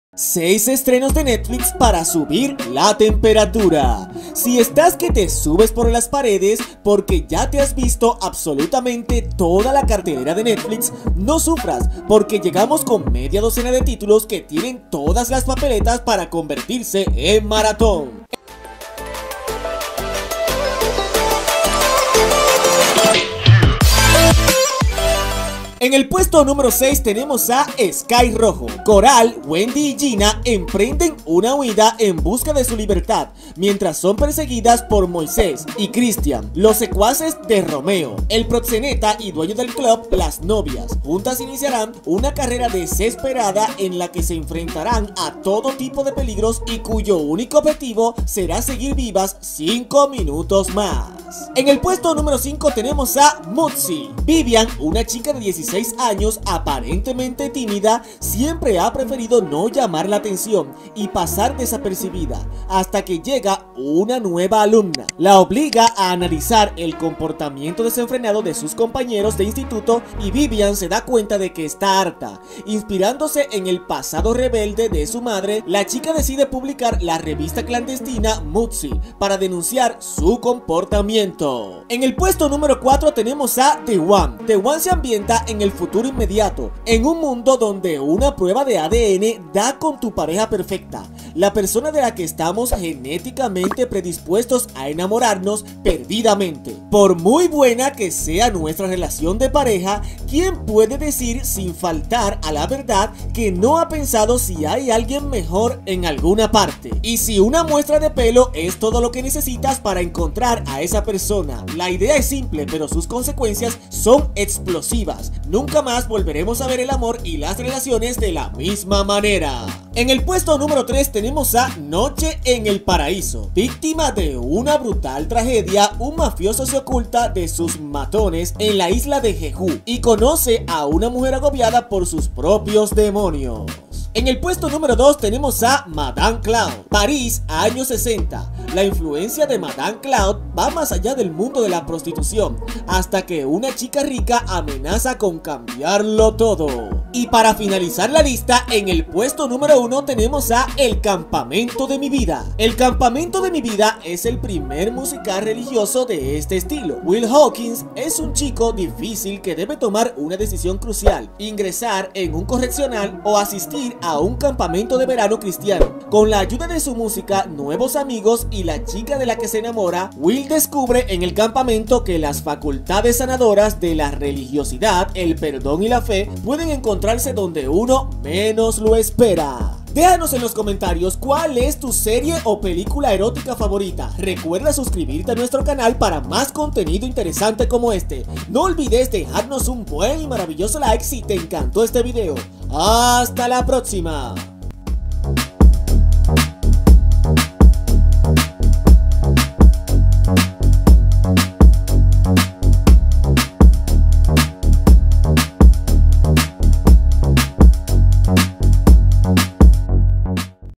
Top 6 Nuevos Estreno Erotico En Netflix
BUENAS GENTE MARAVILLOSA, EN ESTA OCACION LES TRAIGO 6 NUEVAS PRODUCCIONES EROTICAS EN NETFLIX, ESTOS ESTRENOS VAN A SUBIR LA TEMPERATURA HAY PELICULAS Y SERIES INCLUIDAS ESPERO QUE LES GUSTE NO OLVIDEN SUSCRIBIRSE PARA MAS CONTENIDO COMO ESTE, DEJENME UN BUEN LIKE SI LES GUSTO EL VIDEO, HASTA LA PROXIMA!!